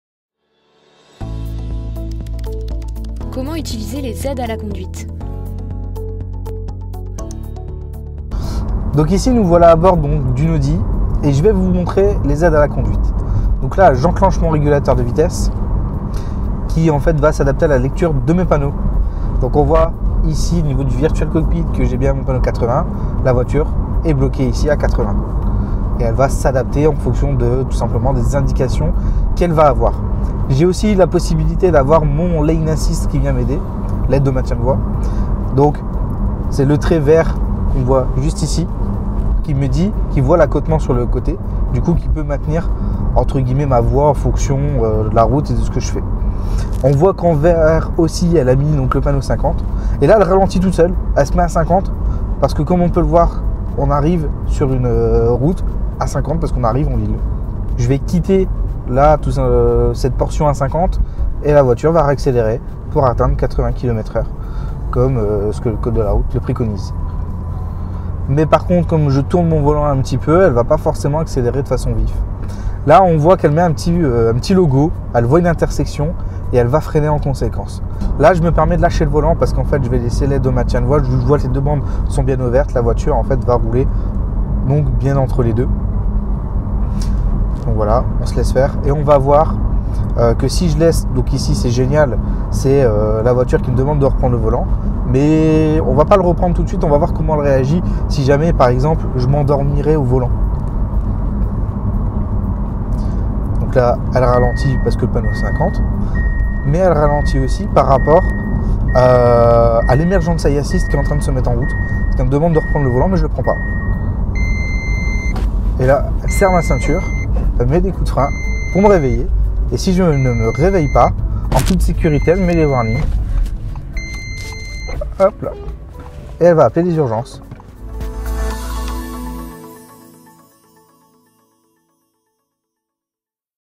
TUTOS - Les aides à la conduite (Audi)
🚗 À quoi servent les aides à la conduite dans votre Audi ? Et comment s'en servir ?
Vous apprendrez avec Jimmy à vous servir du lane assist et découvrir toutes les aides à la conduite les plus technologiques que propose votre Audi.

Vous n'avez plus de raisons de vous endormir au volant !! ❤️